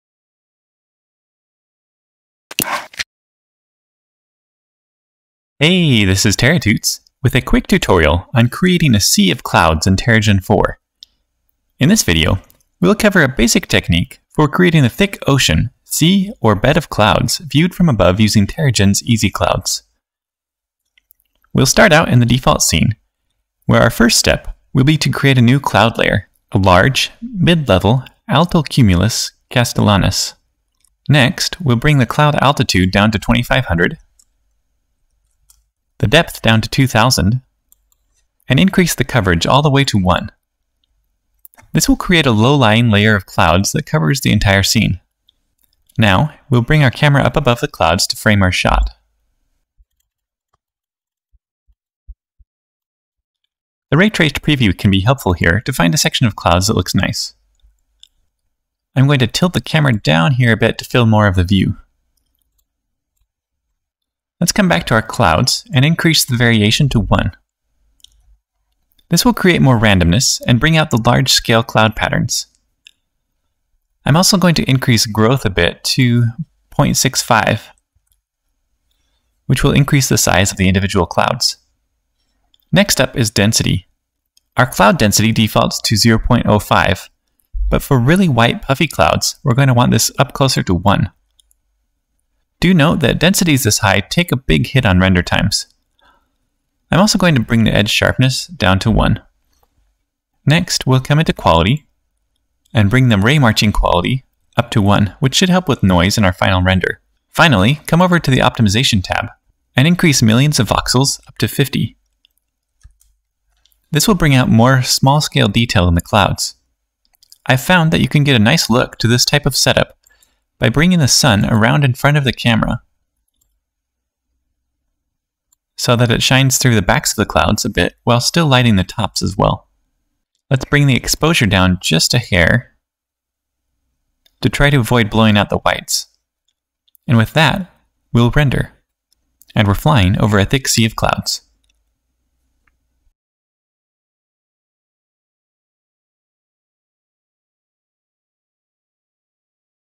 Terragen 4 Basics: Create a Sea of Clouds
A quick introduction to creating an overhead shot of a sea, bed, or ocean of clouds in Terragen.  This video is part of an introductory series on Terragen 4.

Need more?  Check out the new TerraTuts Pro on Udemy!